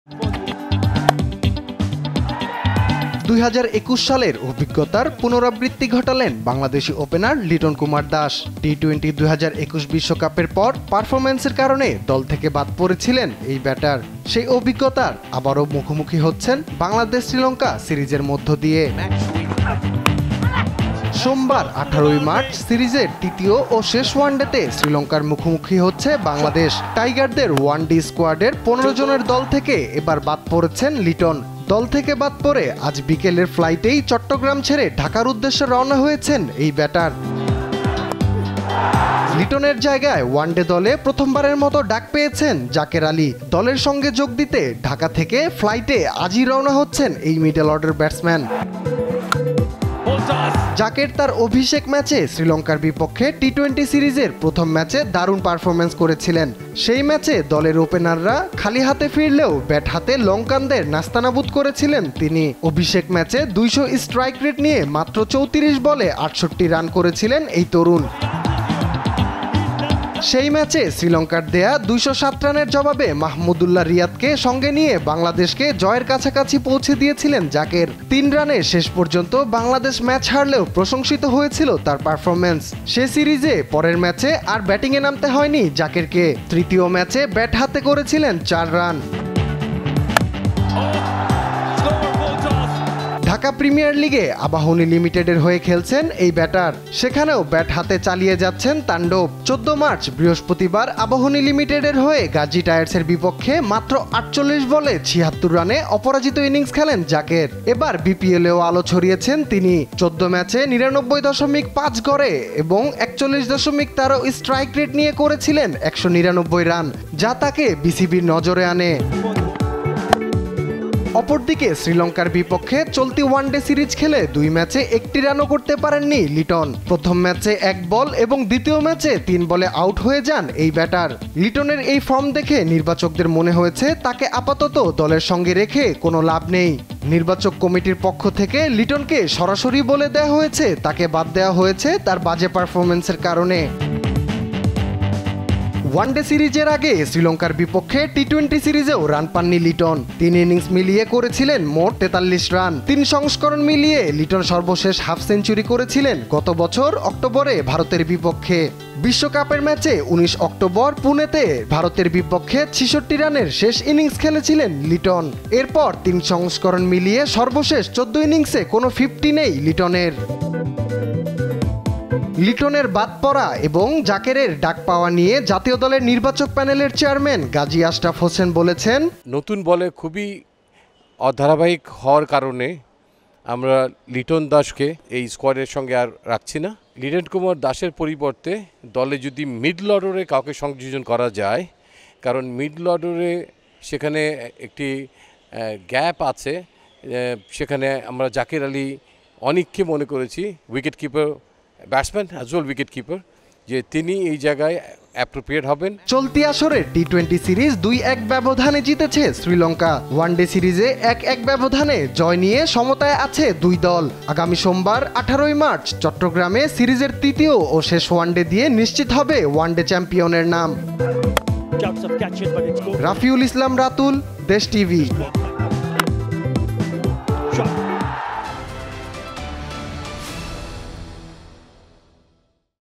কেন বাদ পড়লেন লিটন কুমার দাস? | Liton Das | Jaker Ali | BCB | Sports | News | Desh TV
»» Office Address:
42, Shaheed Sangbadik Selina Parveen Sarak, Malibag 1217 Dhaka, Dhaka Division, Bangladesh